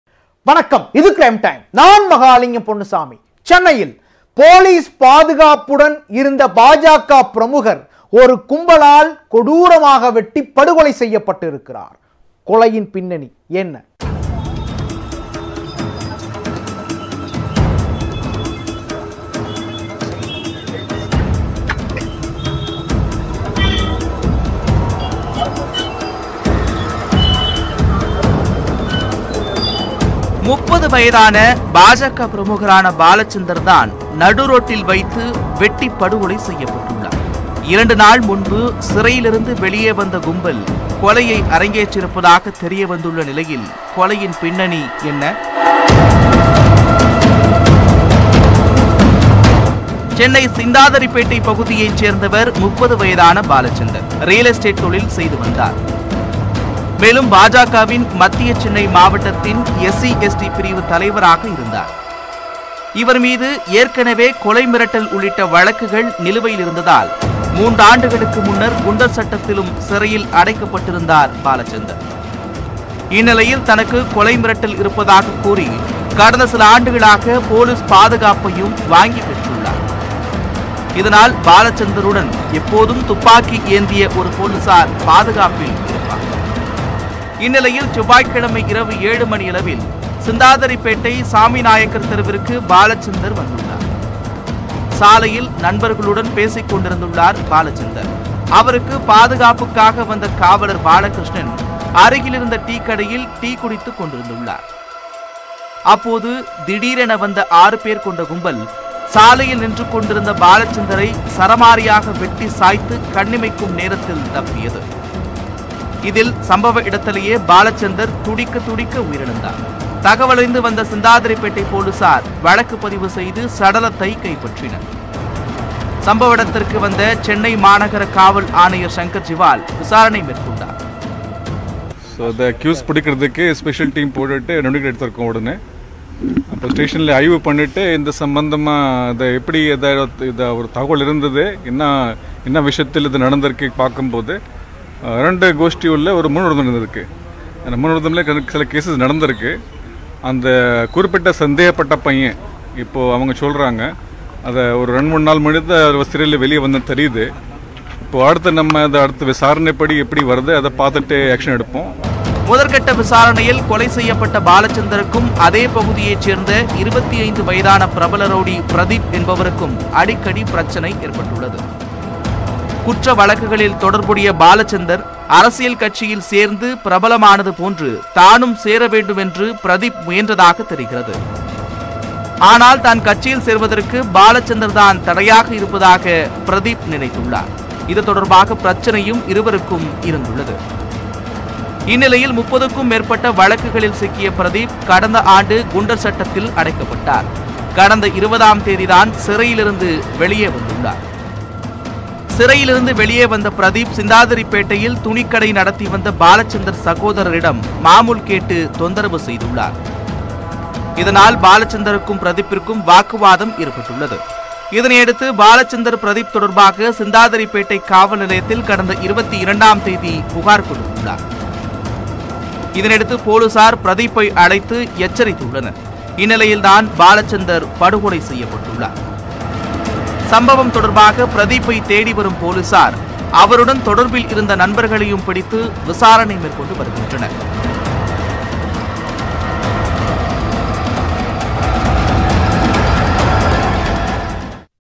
Crime Time | பாஜக பிரமுகர் கொலை - பதவி வெறி காரணமா? | BJP | Balachandar | Chennai
சென்னையில் பா.ஜ.க பிரமுகரை நடு ரோட்டில்வைத்து 6 பேர் கொண்ட கும்பல் வெட்டிக் கொலை செய்துள்ளது.